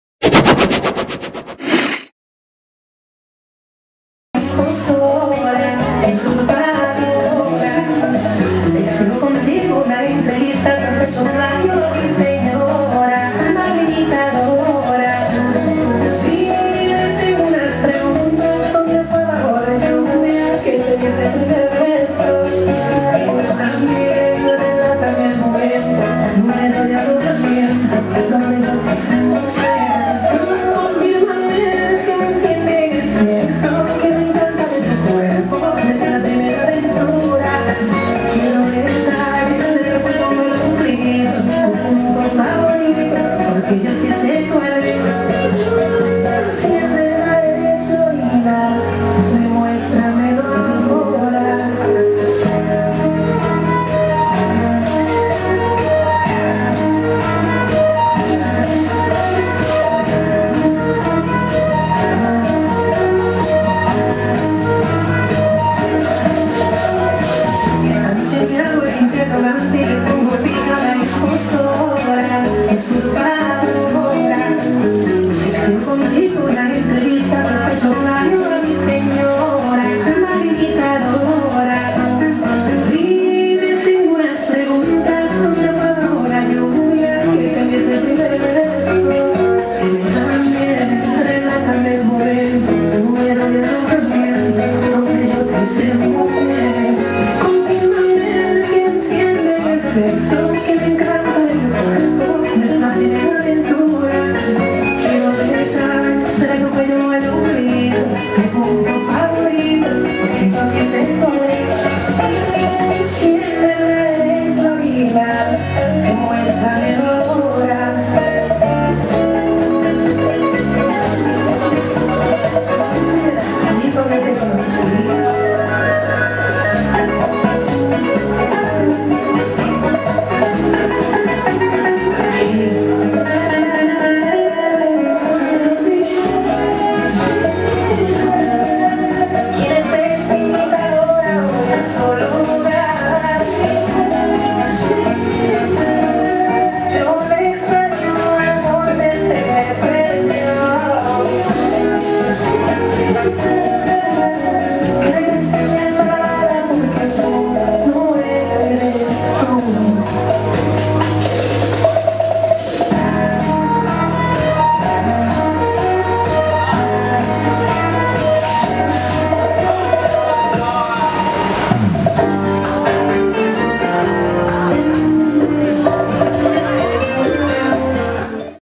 Two blonde women have fun on the Bachata dance floor
2 blonde women have fun in Studio B, Tel-Aviv, Israel.